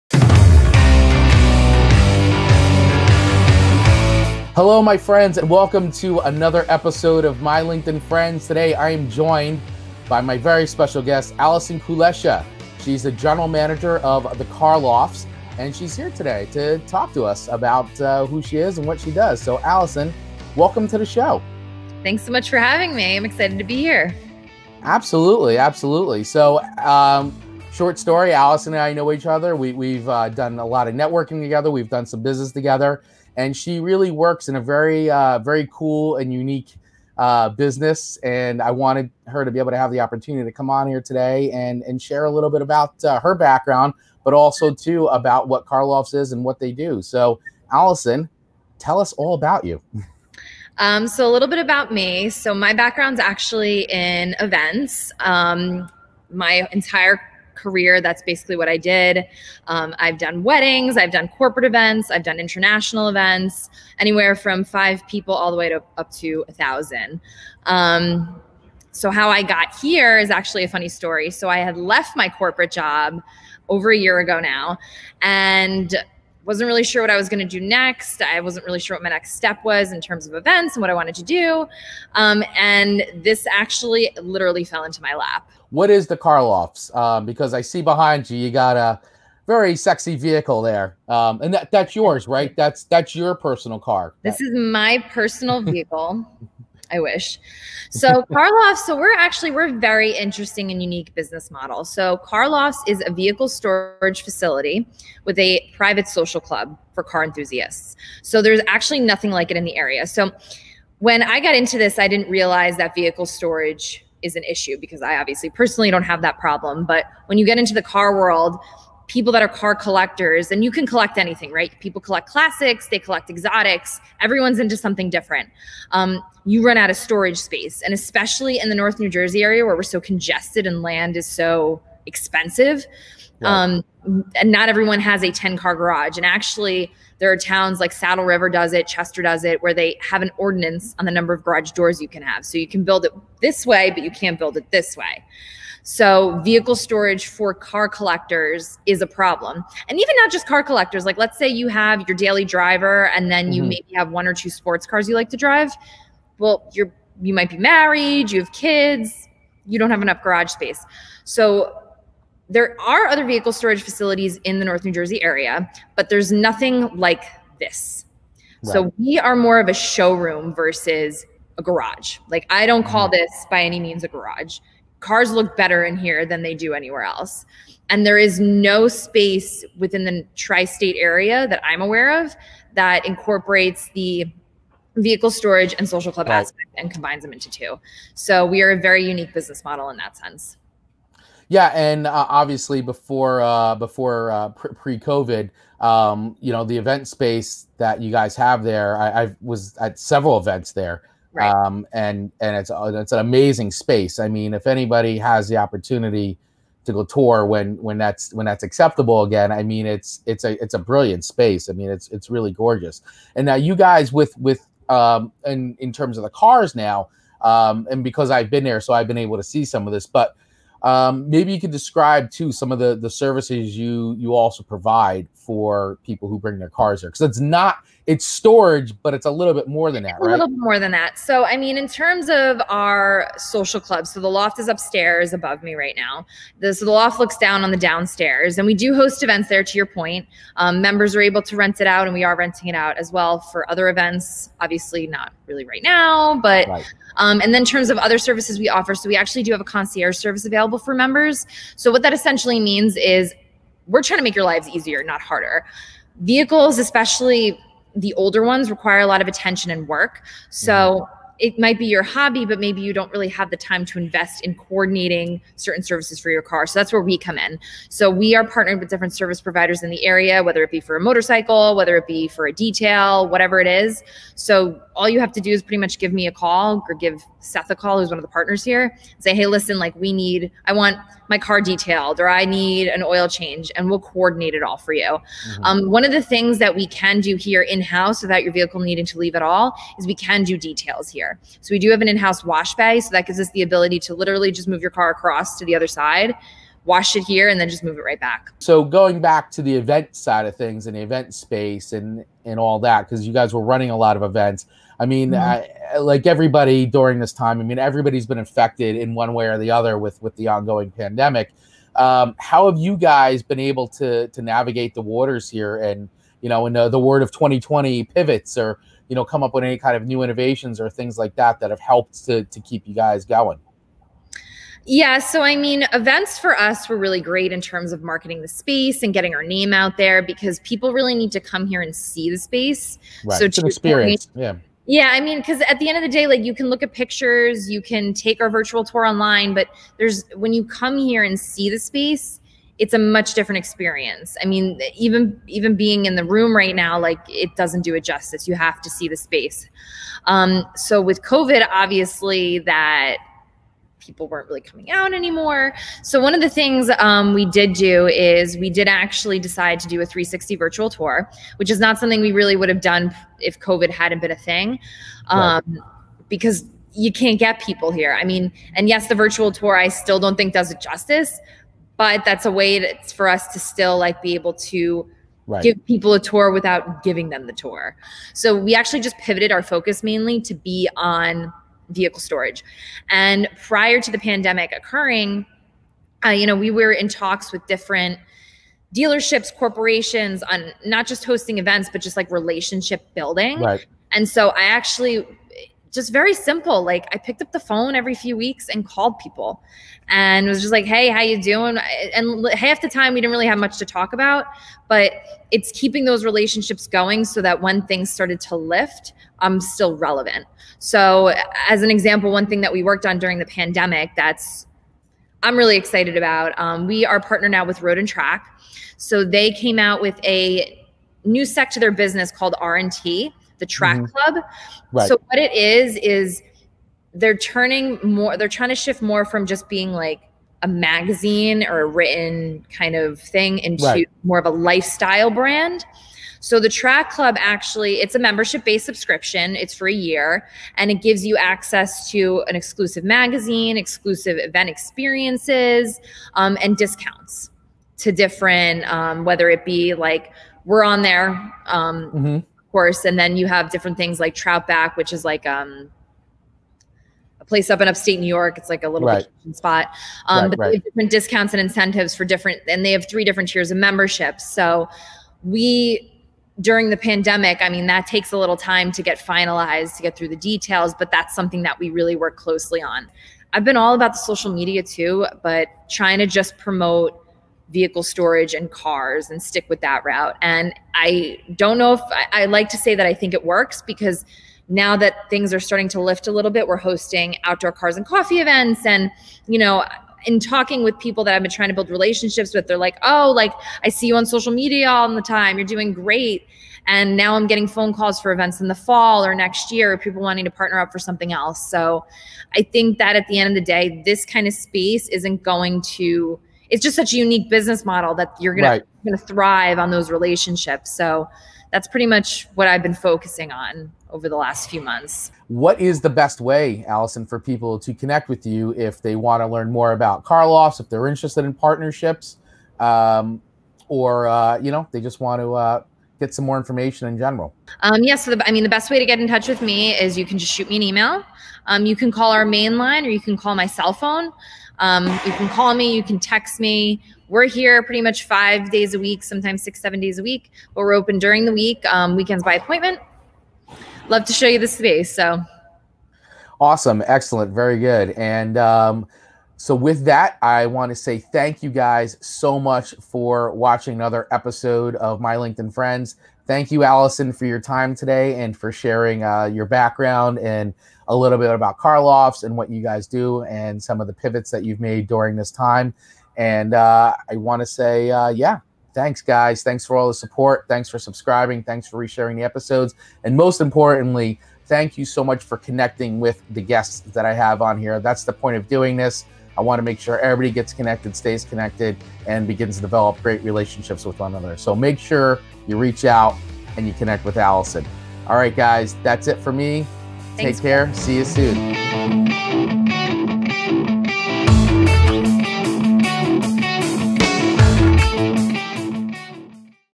My LinkedIn Friends | The Car Lofts | Allison Kulesza
I recently had the opportunity to chat with The Car Lofts General Manager, Allison Kulesza, about her background as an event planner, The Car Lofts, and how they have "pivoted" during the pandemic. If you would like to get more information about The Car Lofts or how to partner with them please connect with Allison or email her at .